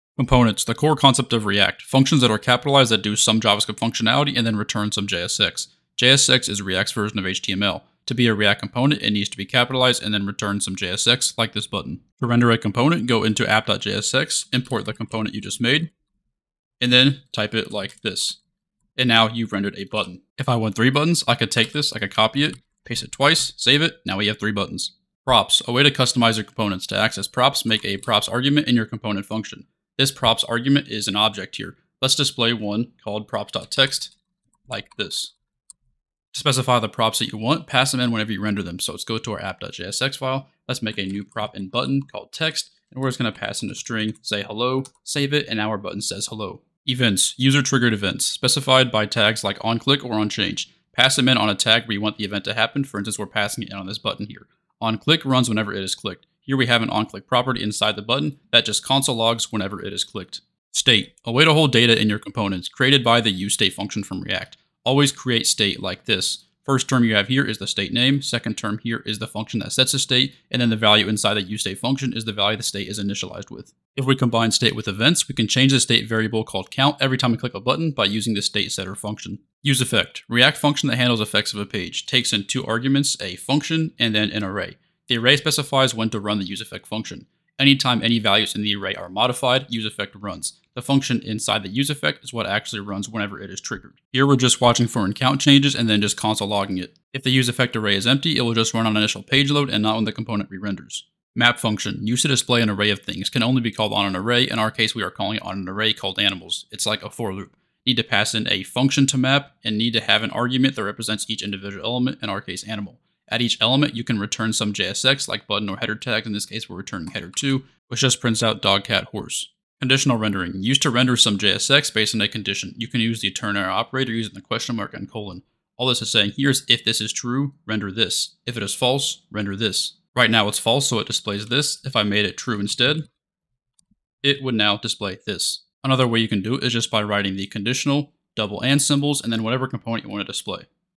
Learn React JS in 2 Minutes
In this very short video you'll learn how to use React and React concepts like components, props, state, events, list rendering, conditional rendering, and mapping.

Become a frontend developer after watching this video!